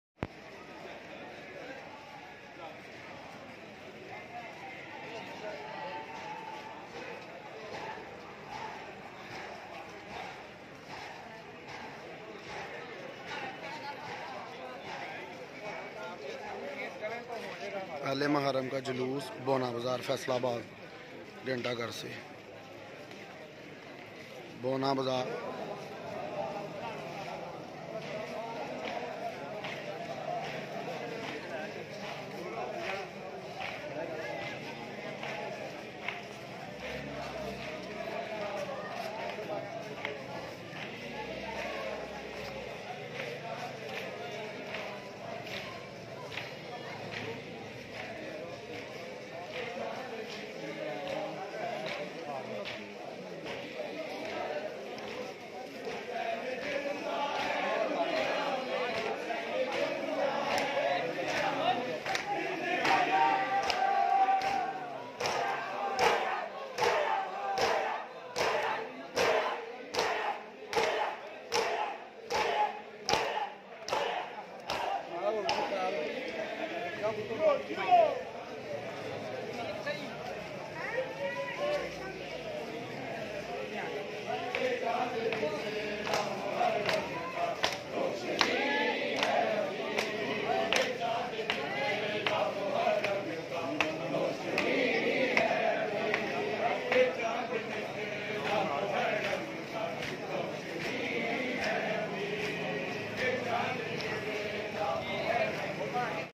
یکم محرم الحرام کے جلوس میں بڑی تعداد میں موجود حیسنی عزادار کے ماتم کرتے ہوئے امام بارگاہ پہنچ گئے
یکم محرم الحرام کے جلوس میں بڑی تعداد میں موجود حیسنی عزادار فیصل آباد کے ماتم کرتے ہوئے واپس امام بارگاہ پہنچ گئے یا حسین یا حسین